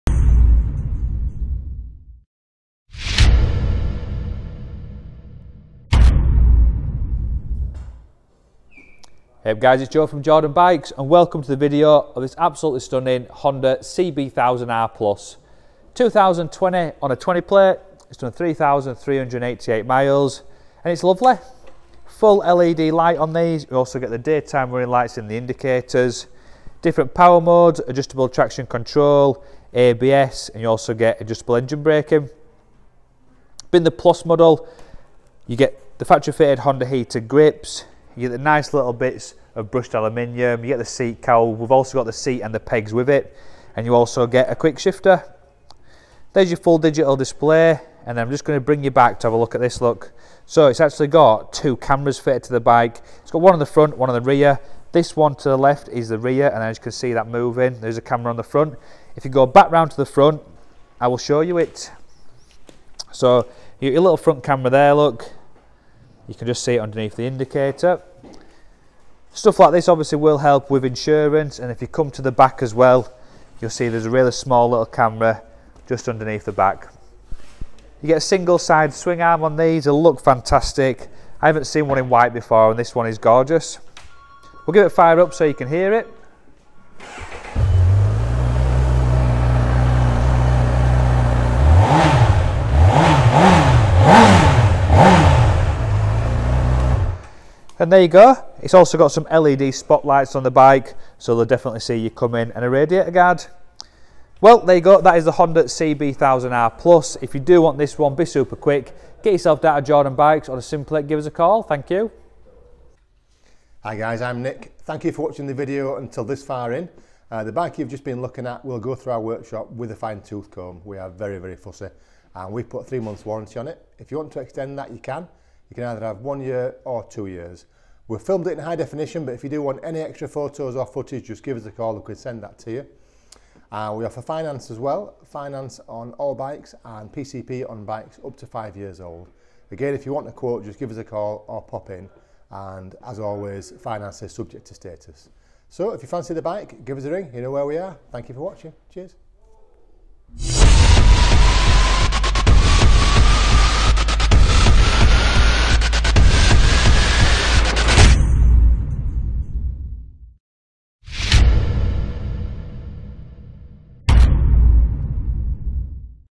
Jordanbikes- For Sale, Honda CB1000R Plus, 2020 20 plate, 3388 miles, £9290
For sale-Honda CB1000R-PLUS, 2020 '20' plate with only 3388 miles on the clock. This is a stunning bike, barely used from new, and being the PLUS model it comes with aluminium inserts down the forks, a factory quickshifter with autoblipper and factory heated grips. These bikes come as standard with Showa big piston forks, Showa rear shock, LED headlights, ABS and rider modes. They are a fantastic road bike offering a claimed 143bhp with fantastic handling and a very stylish single sided swing arm. This particular bike has a front and rear camera system, LED spot lamps, MRA fly screen, seat cowl and rear pegs. What a bike this is for only £9290